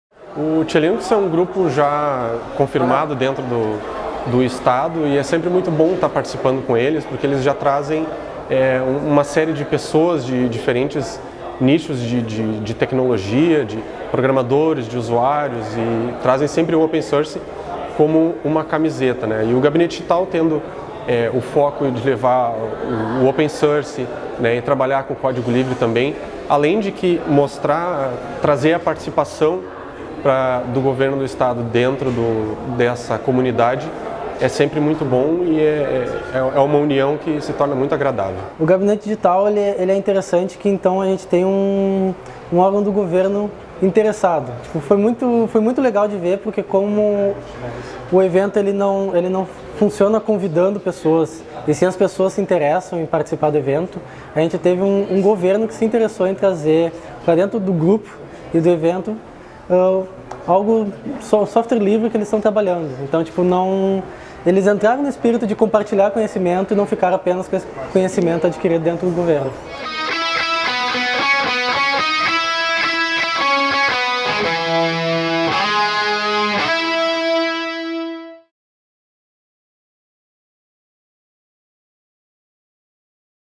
Gabinete Digital é apresentado no TchêLinux
Em mais uma parceria com a comunidade do software livre, o Gabinete Digital foi apresentado em evento do TchêLinux, com oficinas e debates.